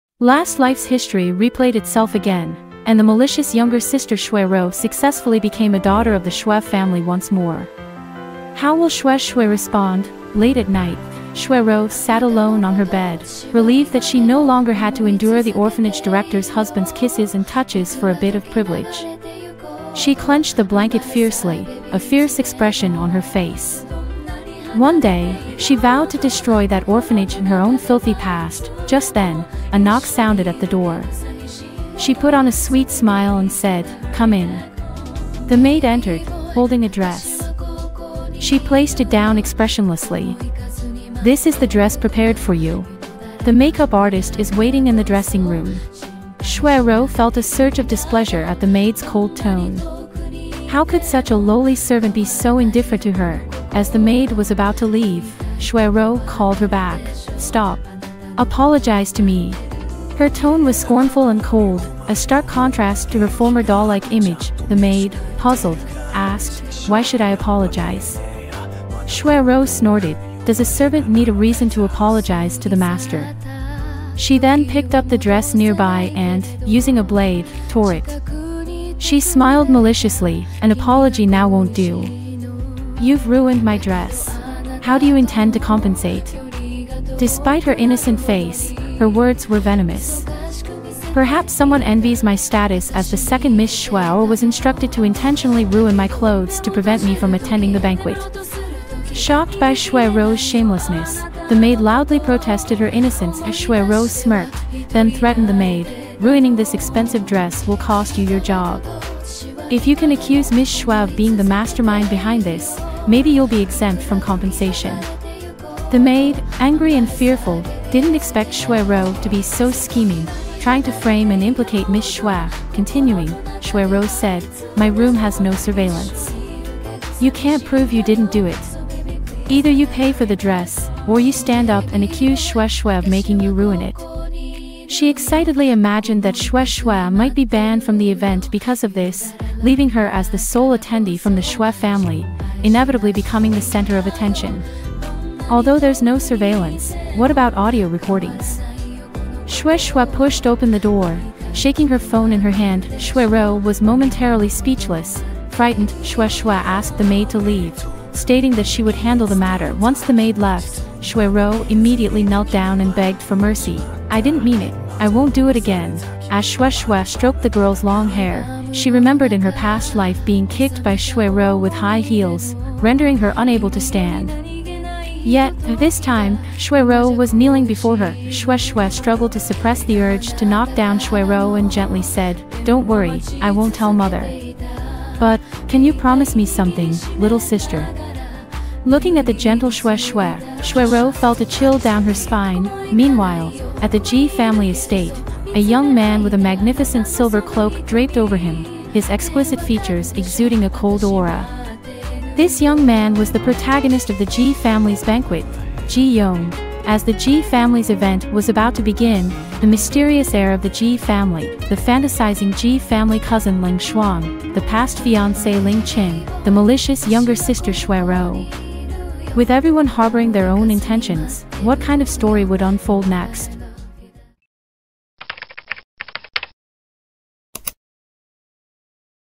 EP12 | Reborn Girl's Revenge: Facing the Ultimate Crisis! - Manhwa Recap
📍 Title: Exclusive Possession: The "Benevolent" Wife
Genre: Romance, Drama
Status: Ongoing
Platform: WebComics App
Sypnosis:
In her previous life, she was conspired against by her family and lover, after which her younger sister and fiancé united in stabbing a knife into her heart. In this life, Xue Xue stands at the top and is no longer be manipulated by others. But why does she provoke this reticent and domineering man just as she takes revenge on her enemies? No matter how she struggled to break free, she can't seem to escape him...